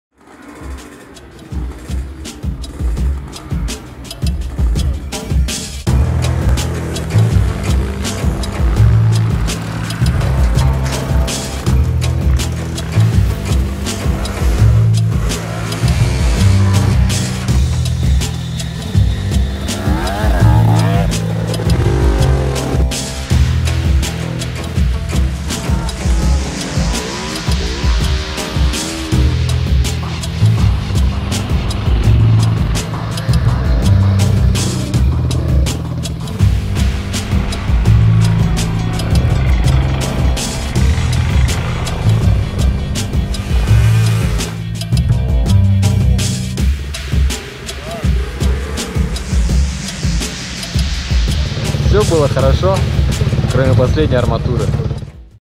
Hard enduro Kamchatka 2017 пролог - анонс.
На Камчатке прошли двух дневные соревнования по эндуро мотоспорту. Выкладываем анонс первого дня гонки! Следите за каналом, скоро выложим полную версию этих "мужских" соревнований по hard-enduro kamchatka 2017.